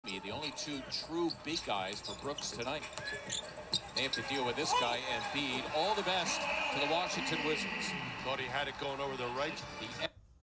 There will always be some magic between Embiid and Simmons
What if Ben Simmons is traded from Philadelphia?